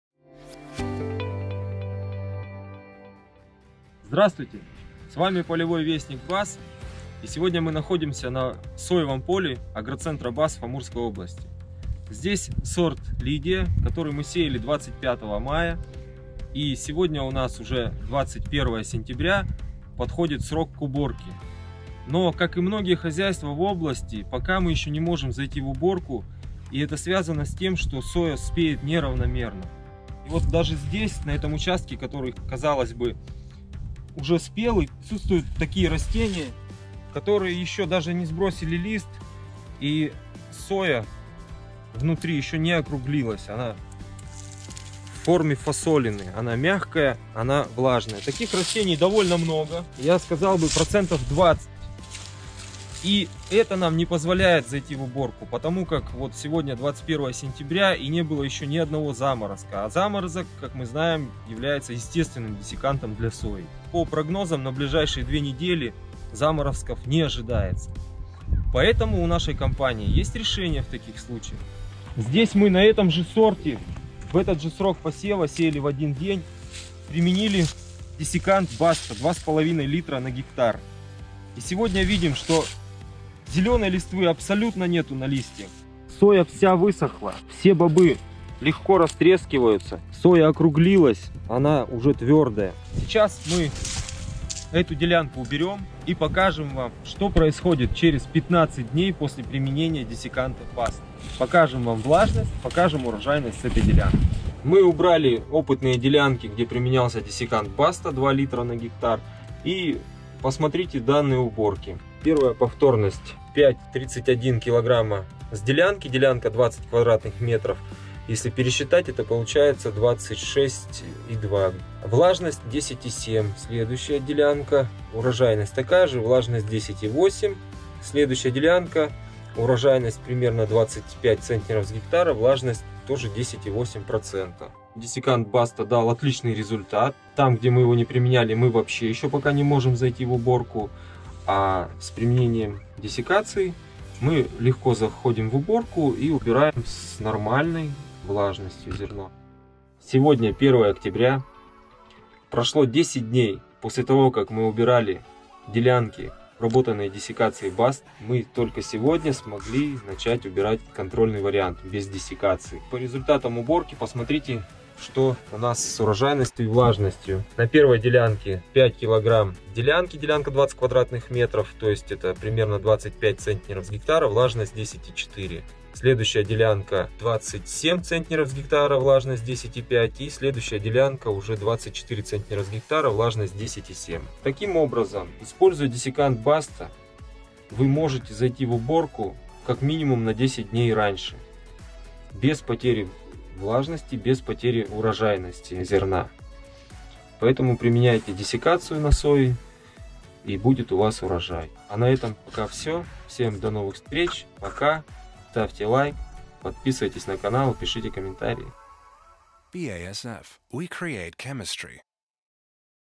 Полевой вестник BASF, выпуск 56 от 19.11.21. Ускорение сбора урожая сои.
Эксперты BASF знают, как ускорить сбор урожая сои без потери его объёма и качества!🌱
Сегодня мы с вами отправимся в АгроЦентр BASF в Благовещенске и посмотрим, как десикант БАСТА® позволяет контролировать равномерность созревания сои.
Внимание на видео!